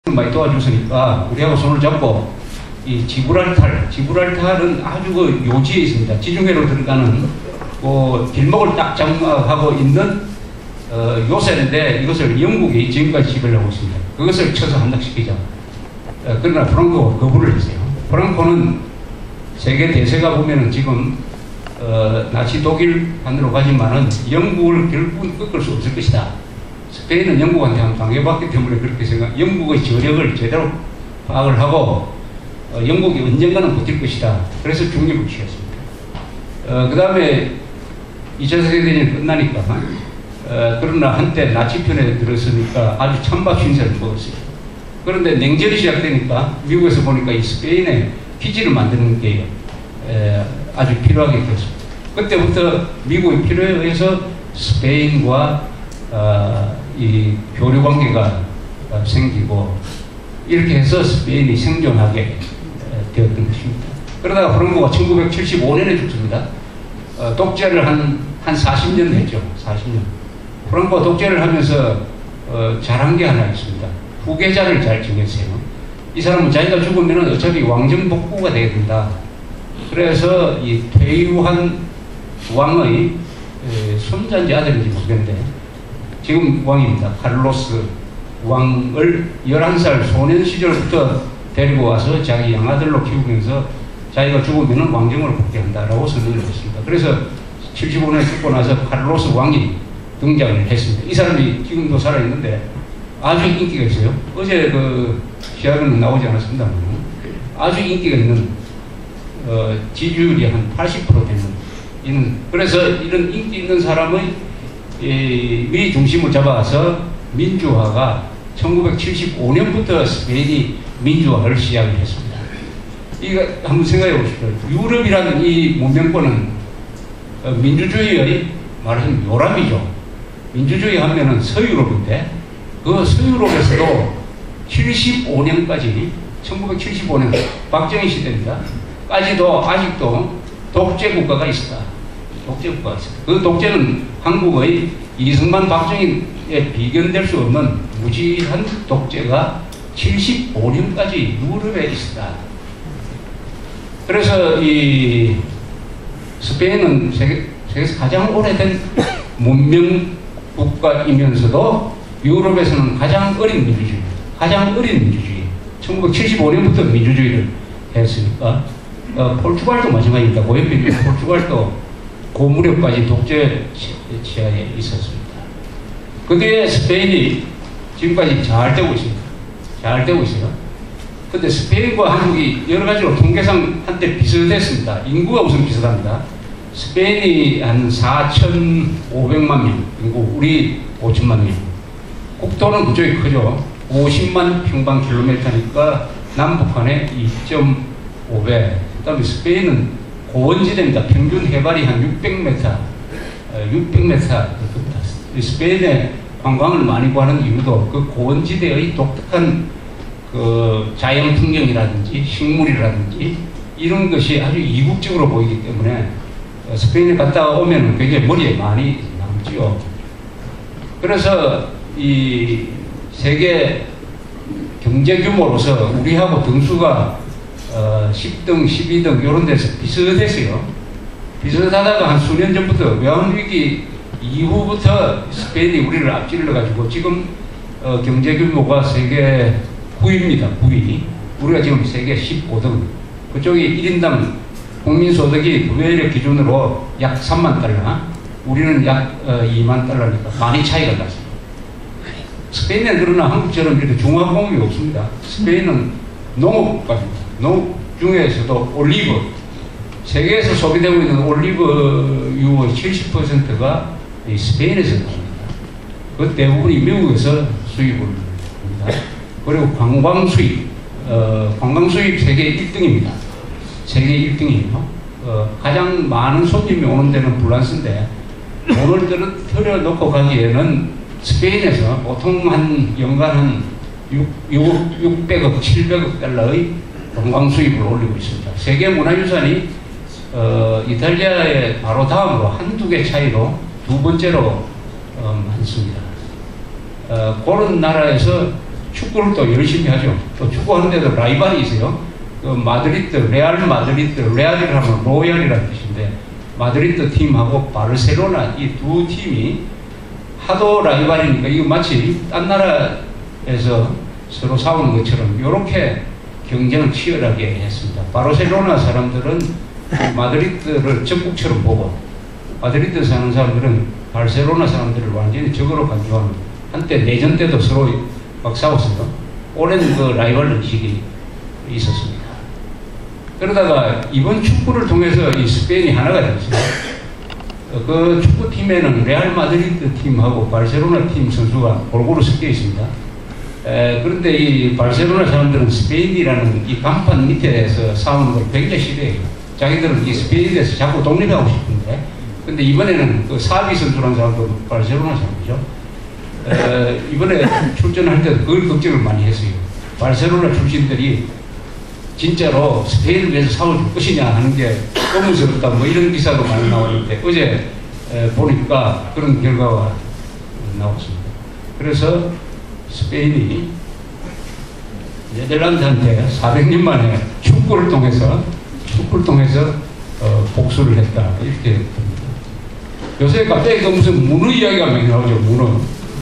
[조갑제 현대사 139-2] 1975년부터 민주화, 세계 9위 경제국
2010년 7월12일 오후 문화일보 홀. 월드컵 결승에서 네델란드를 누르고 우승한 스페인에 대한 이야기.